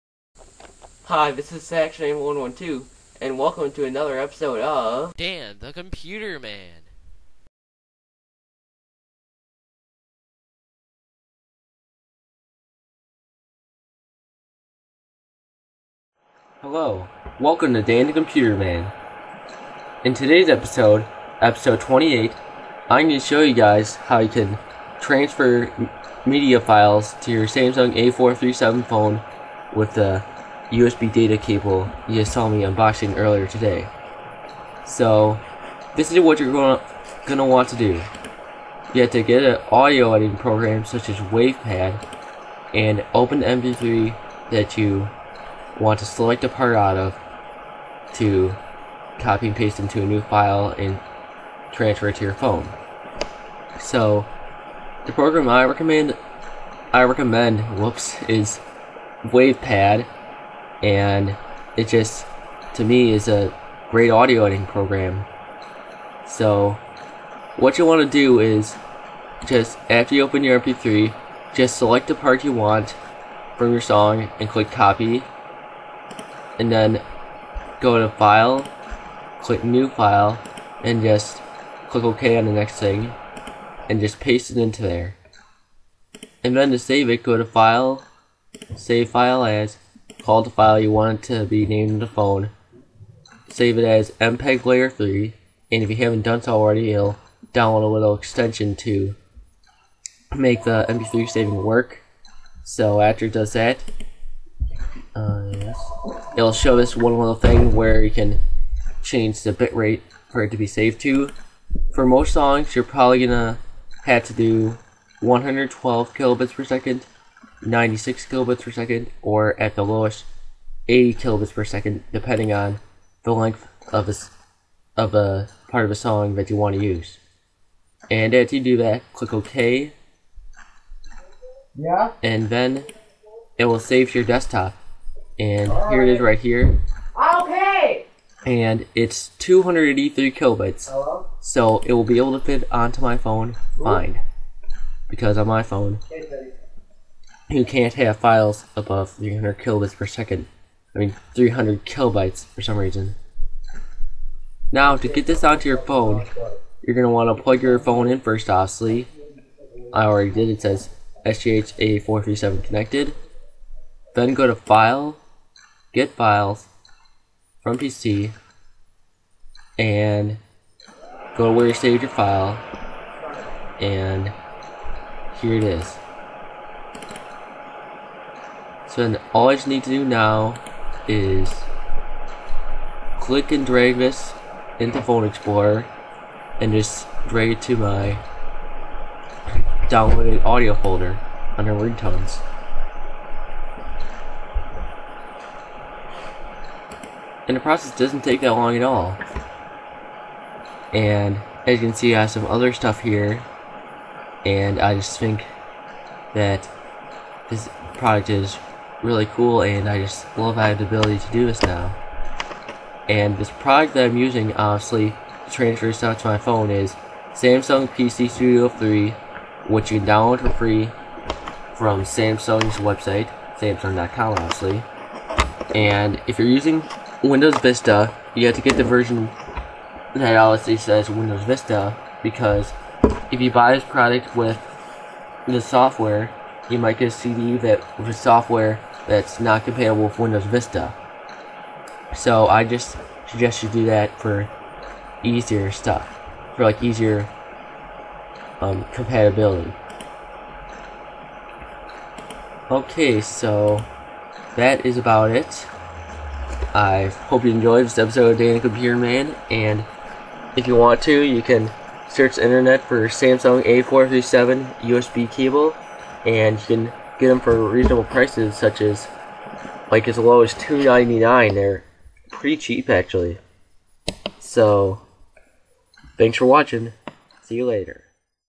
Dan the Computer Man Episode 28
Dan the Computer Man Episode 28 - How to Transfer Audio Files to your Samsung A437 Cell Phone